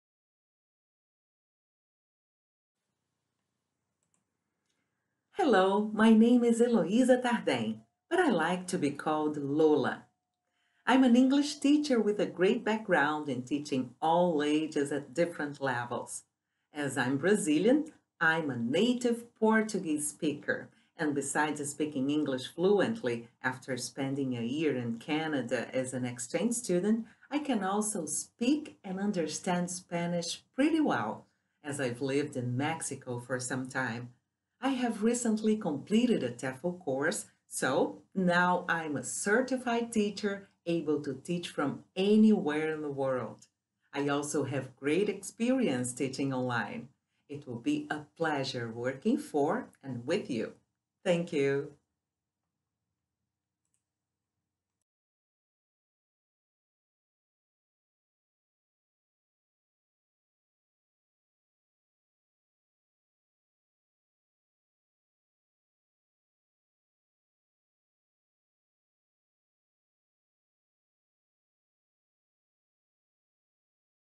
Eloisa Tardem | ITTT Virtual TEFL CV
In this video CV, former ITTT student and TEFL course graduate Eloisa shares her experience and personal qualities in order to find new teaching opportunities worldwide.

If you would like to get in touch with Eloisa, let us know via email web [at] tesol-tefl.net